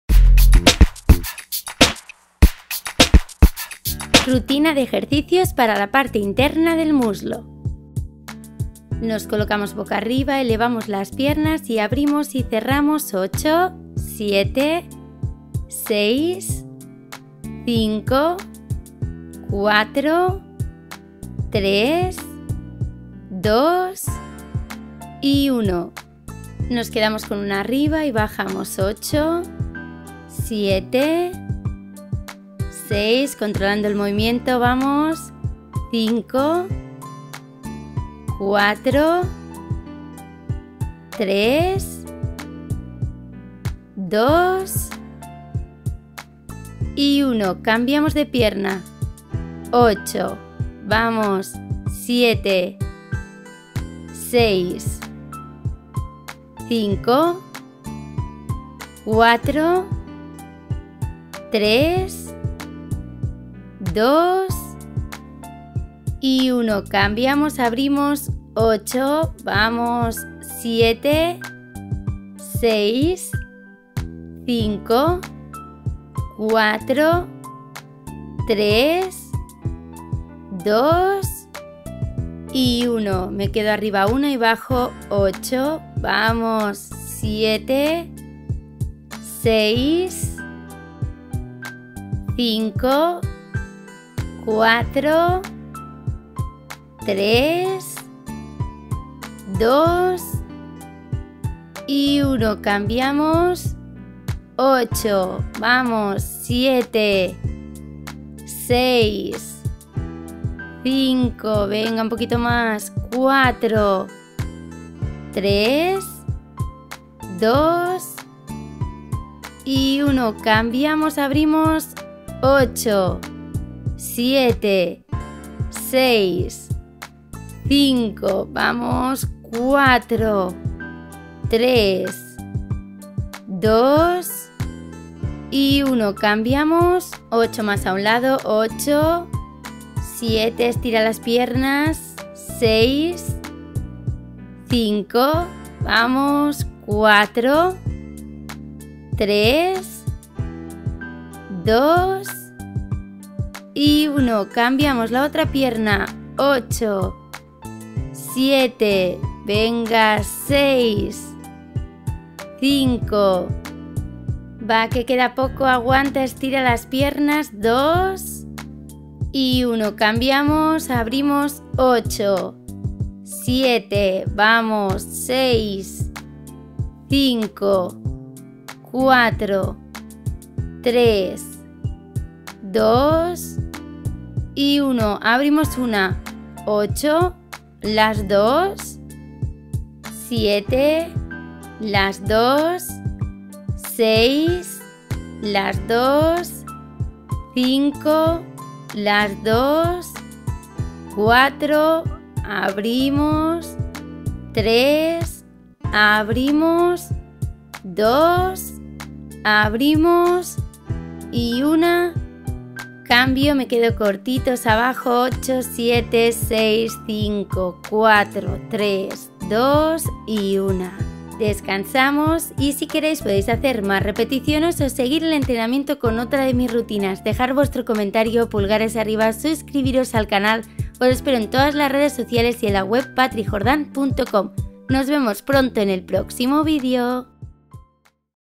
RUTINA EJERCICIOS DE PIERNAS " PARTE INTERNA DEL MÚSLO"
♥ ♥ LEEME / DESPLIEGAME ♥ ♥
RUTINA DE EJERCICIOS DE PIERNAS PARTE INTERNA DEL MÚSLO

Una rutina para tonficar la parte interna del múlso y piernas que podéis realizar 3 o 4 veces por semana para trabajar el gúteos en casa en unos minutos. Podéis unir esta rutina a otras rutinas de tonificación que podéis encontrar en el canal.

AQUÍ UNA RUTINA QUE PODÉIS HACER EN CASA.

Podéis hacer los ejercicios mirando el vídeo 3 o 4 días a la semana

Ire subiendo más ejercicios para que podáis hacer como rutina mirando el vídeo!
Espero que os sea de ayuda, Dejar vuestro comentario !!